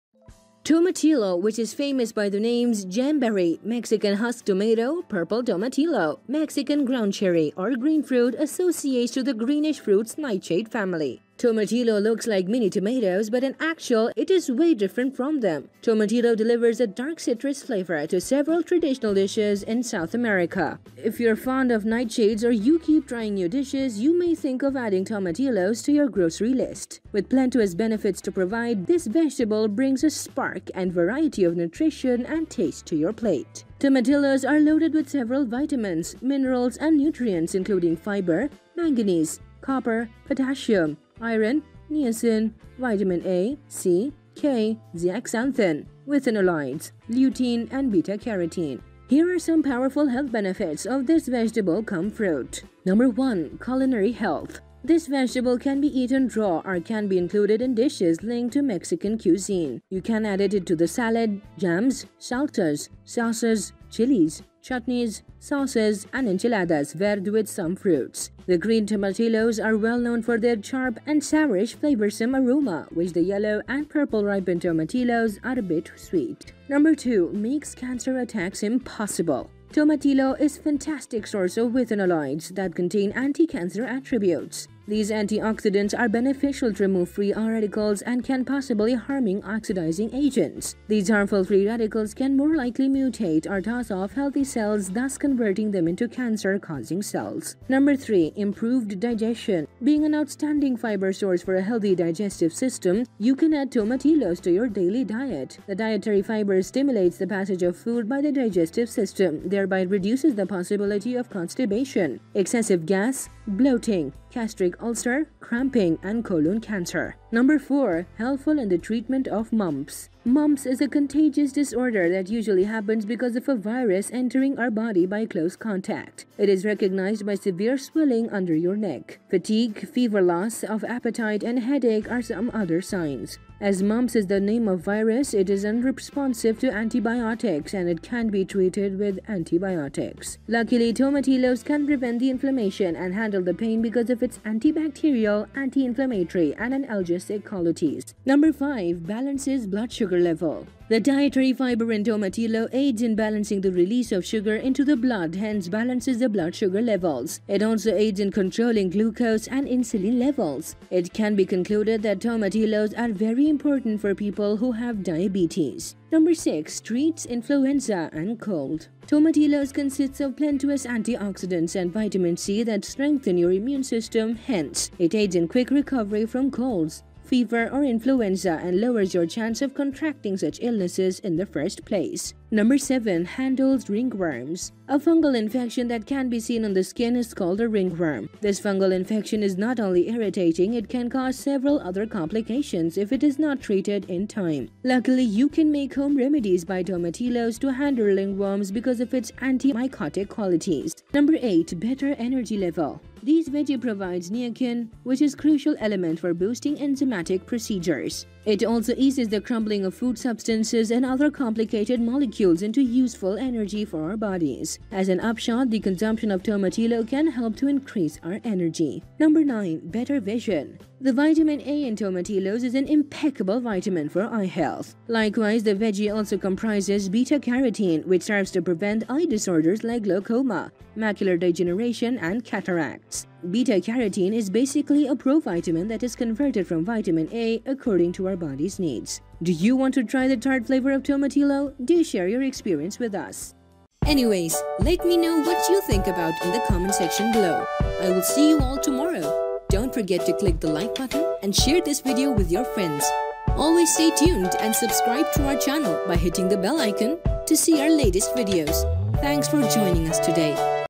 9 Tomatillo Health Benefits for Your Health
9 Tomatillo Health Benefits for Your Health

Tomatillo, which is famous by the names; Jamberry, Mexican husk tomato, purple tomatillo, Mexican groundcherry, or green fruit, associates to the greenish fruits nightshade family. Tomatillo looks like mini-tomatoes, but in actual, it is way different from them. Tomatillo delivers a dark citrus flavor to several traditional dishes in South America.
If you're fond of nightshades or you keep trying new dishes, you may think of adding tomatillos to your grocery list. With plenteous benefits to provide, this vegetable brings a spark and variety of nutrition and tastes to your plate.
Tomatillos are loaded with several vitamins, minerals, and nutrients, including fiber, manganese, copper, potassium, iron, niacin, vitamin A, C, K, zeaxanthin, withanolides, lutein, and beta carotene.

Here are some powerful health benefits of this vegetable cum fruit.

1) Culinary health
This vegetable can be eaten raw or can be included in dishes linked to Mexican cuisine. You can add it to the salad, jams, salsas, sauces, chilis, chutneys, sauces, and the enchiladas Verde with some fruits. The green tomatillos are well-known for their sharp and sourish flavorsome aroma, while the yellow and purple ripened tomatillos are a bit sweet.

2) Makes cancer attacks impossible
Tomatillo is a fantastic sourced of withanolides (an antioxidant) that contains anti-cancer attributes. These antioxidants are beneficial to remove free radicals and any possibly harming oxidizing agents. These harmful free radicals can more likely mutate or toss off healthy cells, thus converting them into cancer, causing cells.

3) Improved digestion
Being an outstanding fiber source for a healthy digestive system, you can add tomatillos to your daily diet. The dietary fiber stimulates the passage of food by the digestive system, thereby reduces the possibility of constipation, excessive gas, bloating, gastric ulcer, cramping, and colon cancer.

4) Helpful in the treatment of mumps
Mumps is a contagious disorder that usually happens because of a virus entering our body by close contact. It is recognized by severe swelling under your neck. Fatigue, fever, loss of appetite, and headache are some other signs. As mumps is the name of a virus, it is unresponsive to antibiotics, and it can't be treated with antibiotics.
Luckily tomatillos can prevent the inflammation and handle the pain because of its antibacterial, anti-inflammatory, and analgesic qualities.

5) Balances blood sugar level
The dietary fiber in tomatillo aids in balancing the release of sugar into the blood hence balances the blood sugar levels. It also aids in controlling glucose and insulin levels. It can be concluded that tomatillos are very important for people who have diabetes.

6) Treat influenza and cold
Tomatillos consist of plenteous antioxidants and vitamin C that strengthen your immune system; hence, it aids in quick recovery from colds, fever, or influenza and lowers your chance of contracting such illnesses in the first place.

7) Handles ringworms
A fungal infection that can be seen on the skin is called ringworm. This fungal infection is not only irritating; it can cause several other complications if it is not treated in time. Luckily, you can make home remedies by tomatillos to handle ringworms because of its anti-mycotic qualities.

8) Better energy level
This veggie provides niacin, which is a crucial element for boosting enzymatic procedures. It also eases the crumbling of food substances and other complicated molecules into useful energy for our bodies. As an upshot, the consumption of tomatillo can help to increase our energy.

9) Better vision
The vitamin A in tomatillos is an impeccable vitamin for eye health. Likewise, the veggie also comprises beta-carotene, which serves to prevent eye disorders like glaucoma, macular degeneration, and cataracts. Beta-carotene is basically a pro-vitamin that is converted from vitamin A according to our body's need.
Do you want to try the tart flavor of tomatillo? Do share your experience with us!